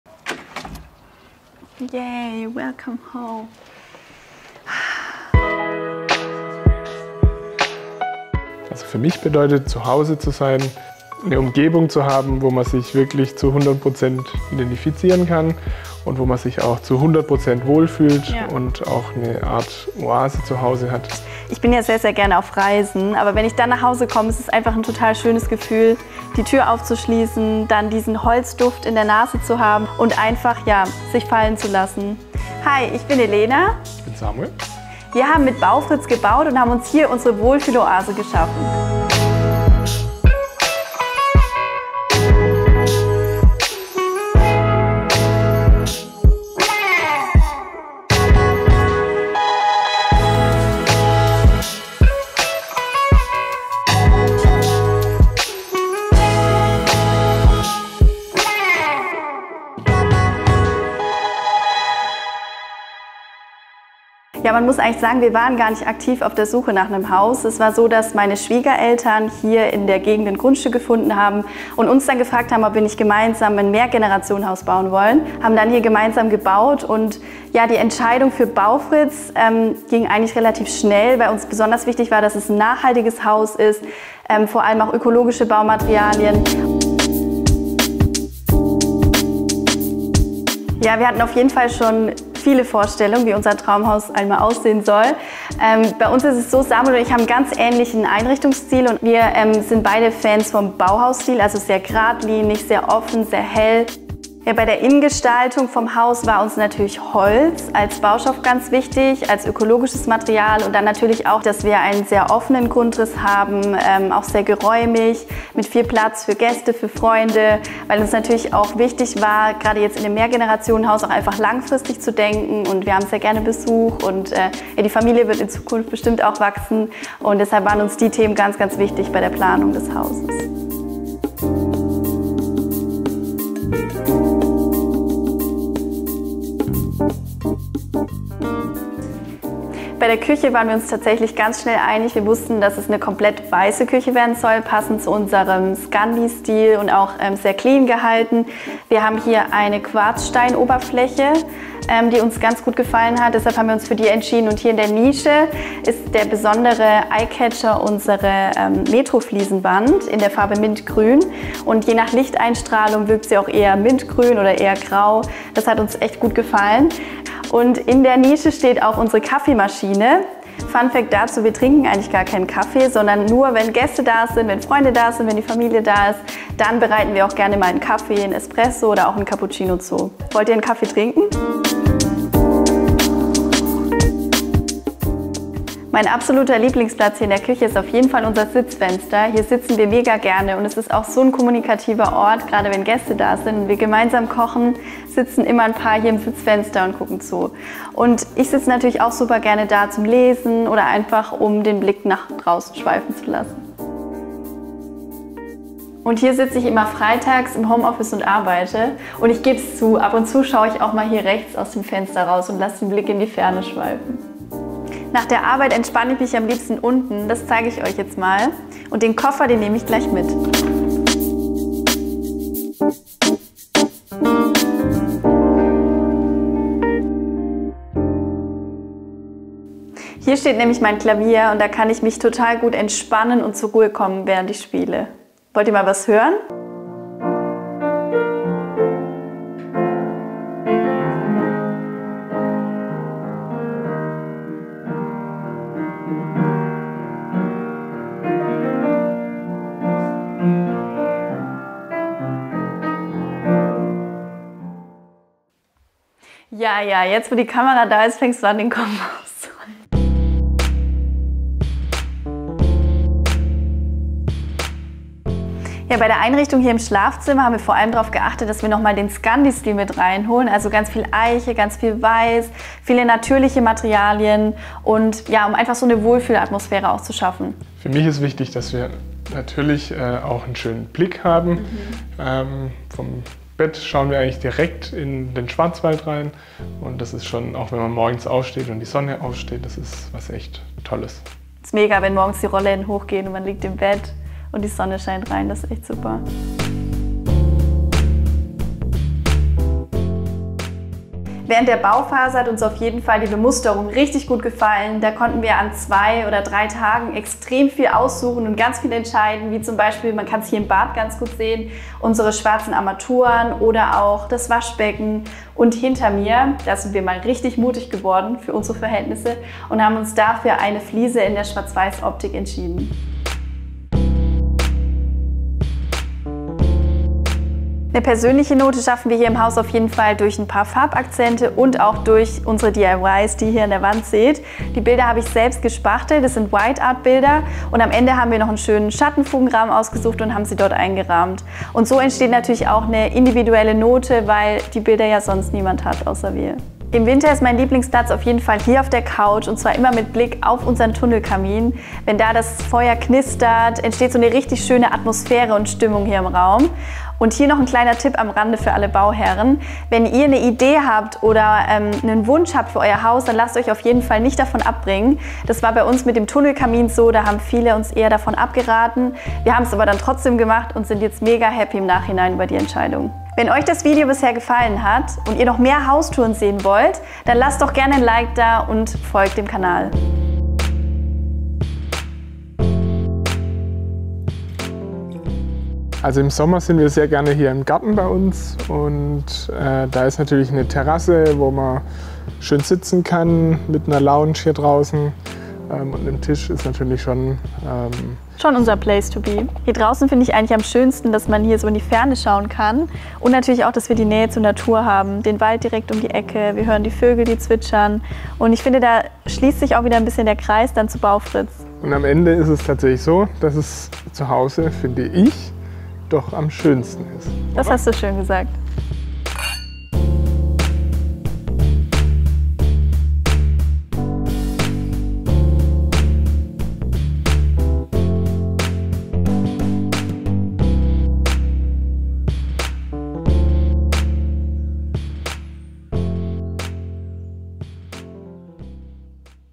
MEHRGENERATIONENHAUS im SKANDI STYLE | Elenas und Samuels @baufritz Haus | Haustour
Das größte Hobby von Elena und Samuel ist das Reisen. Doch heute nehmen sie uns mit in ihr Zuhause im Schwarzwald. Dort haben sie mit @baufritz ein Mehrgenerationenhaus gebaut.

Im großen Wohn-Essbereich mit offener Küche werden wir vom cleanen skandinavischen Look begrüßt. Ein besonderes Highlight: der Tunnelkamin. Im Untergeschoss ziehen sich Elena und Samuel zurück, entspannen beim Klavierspielen, lassen sich von der über dem Schwarzwald aufgehenden Sonne wecken und genießen die Panoramasicht von ihrer Terrasse aus.

Elena und Samuel haben den perfekten Mix aus ökologisch und modern geschafft und beweisen, dass ein Fertighaus komplett individuell gestaltet werden kann.